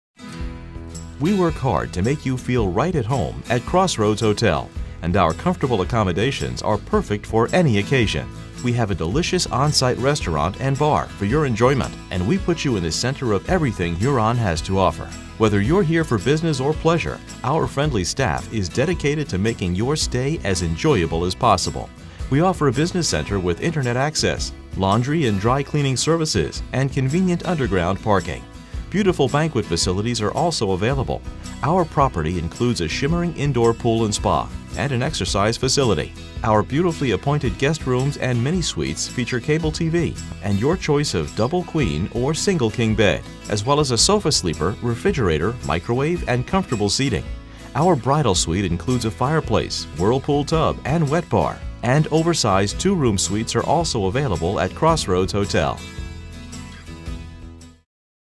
Crossroads Hotel - Huron, SD
We work hard to make you feel right at home at Crossroads Hotel, and our comfortable accommodations are perfect for any occasion.  We have a delicious on-site restaurant and bar for your enjoyment, and we put you in the center of everything Huron has to offer.

We offer a business center with internet access, laundry and dry cleaning services, and convenient underground parking.  Beautiful banquet facilities are also available.  Our property includes a shimmering indoor pool and spa, and an exercise facility.

Our beautifully appointed guest rooms and mini-suites feature cable TV, and your choice of double queen or a single king bed, as well as a sofa sleeper, refrigerator, microwave, and comfortable seating.  Our bridal suite includes a fireplace, whirlpool tub, and wet bar, and oversized two-room suites are also available at Crossroads hotel.